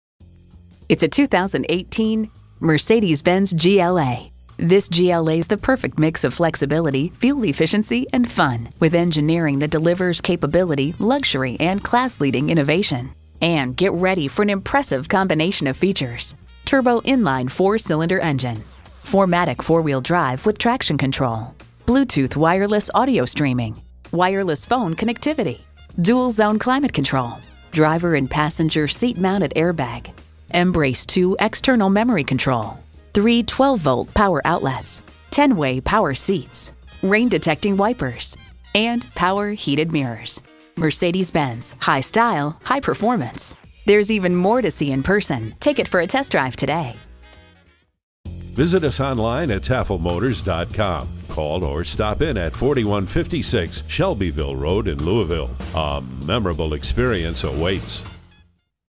2018 Mercedes-Benz GLA Louisville KY Elizabethtown, KY US11495 - SOLD
Year: 2018
Make: Mercedes-Benz
Model: GLA
Trim: GLA 250 4MATIC SUV
Engine: 4 Cyl. 2.0 L
Transmission: Automatic
Color: Jupiter Red
Interior: Black
Mileage: 3989
Stock #: US11495
VIN: WDCTG4GB7JJ457160
P Clean CARFAX. Jupiter Red 2018 Mercedes-Benz GLA GLA 250 4MATICA® 4MATICA® 7-Speed Automatic 2.0L I4 DI TurbochargedRecent Arrival! 31/23 Highway/City MPG

Address:
4156 Shelbyville Road